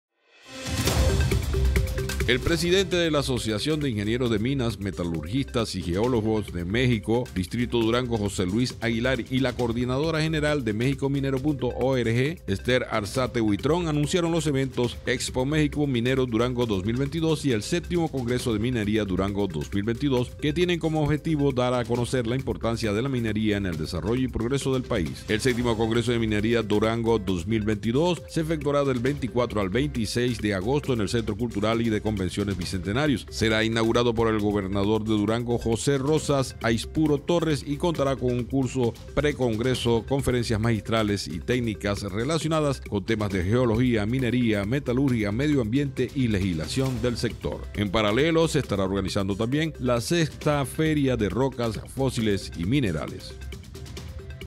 VII Congreso de Minería Durango y Expo México Minero Durango 2022 inician esta semana 👥
El VII Congreso de Minería Durango 2022, se efectuará del 24 al 26 de agosto en el Centro Cultural y de Convenciones Bicentenario, será inaugurado por el gobernador de Durango, José Rosas Aispuro Torres, y contará con un curso precongreso, conferencias magistrales y técnicas relacionadas con temas de geología, minería, metalurgia, medio ambiente y legislación del sector. En paralelo, se organizará la Sexta Feria de Rocas, Fósiles y Minerales.
Fuente: MundoMinero.mx

Inspenet, ¡Tu red de conocimiento profesional!